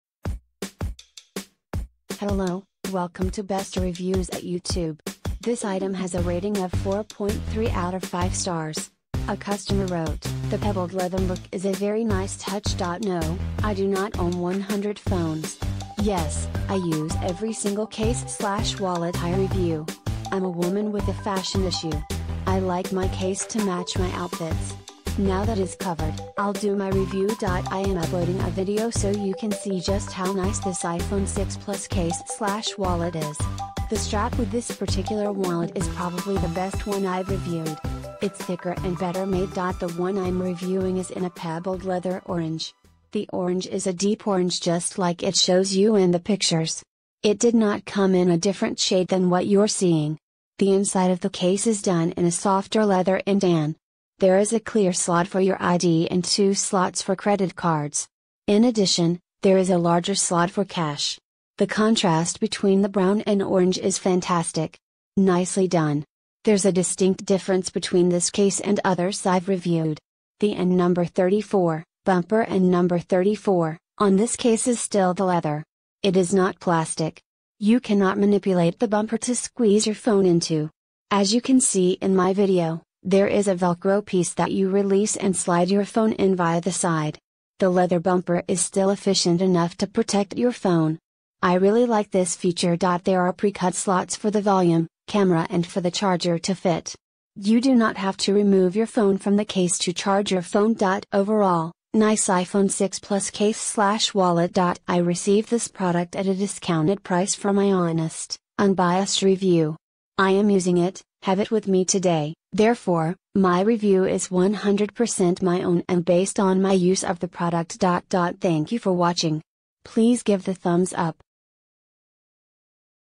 iPhone 6s Plus (5.5) Cases, iPhone 6 Plus (5.5) [Snow Fairy] Flip Cover Case [Premium Genuine
Review about   iPhone 6s Plus (5.5) Cases, iPhone 6 Plus (5.5) [Snow Fairy] Flip Cover Case [Premium Genuine Leather Kick Stand Series][Card Holder][Wallet] - [Leather Fit][Wrist Strap] Bull Leather Case Purple